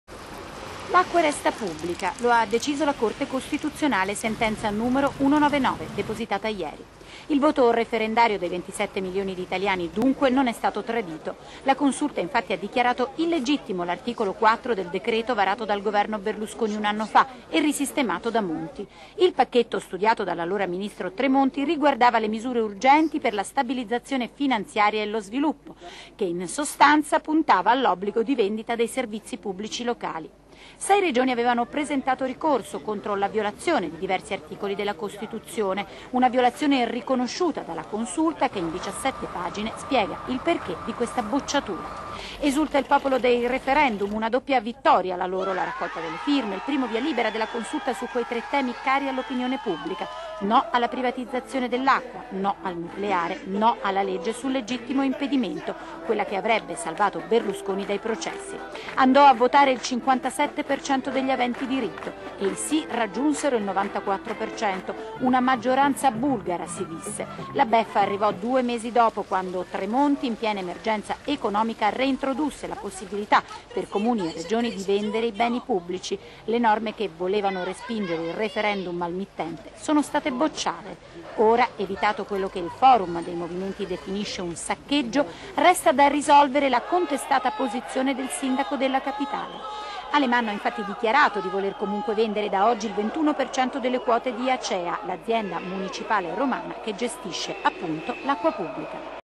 Consulta, l'acqua resta pubblica
Consulta, l'acqua resta pubblica. La Corte Costituzionale boccia la privatizzazione dei servizi pubblici prevista dal governo Berlusconi. Esulta il Popolo dell'acqua, vincitore del referendum.
Servizio di Roberta Serdoz